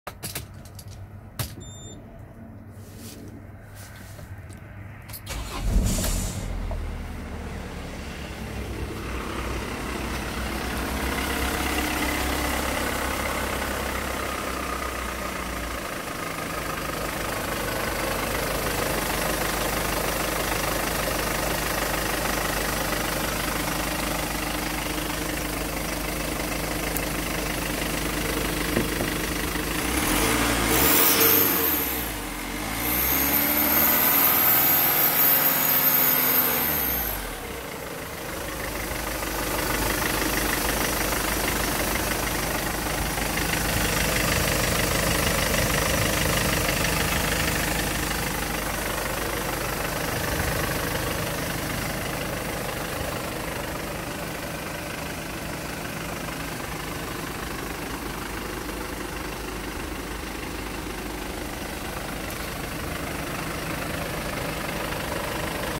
Motor Mercedes Sprinter 2.2 CDI Euro 5 2010 - 2016
SDG Auto Dezmembrări Oradea vinde Motor Mercedes Sprinter 2.2 CDI Euro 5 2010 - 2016.
Piesa se prezinta in stare buna.